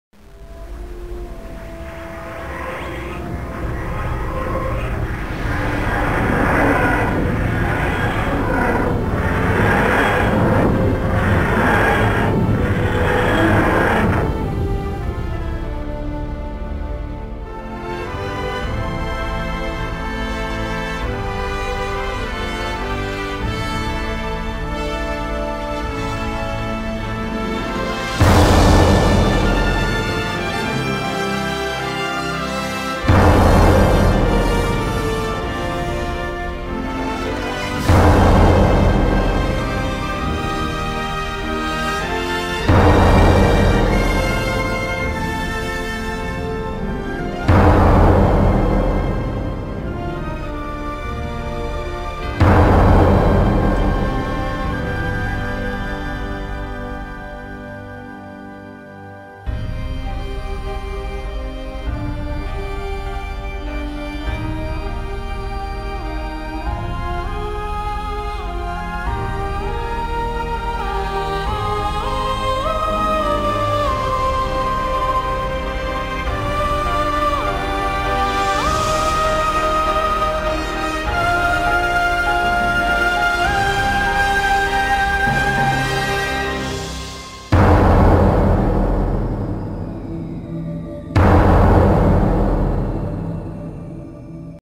TIME Busters - The Biblios Diary - Main Teaser
A little teaser to my upcoming project 'TIME Busters - The Biblios Diary'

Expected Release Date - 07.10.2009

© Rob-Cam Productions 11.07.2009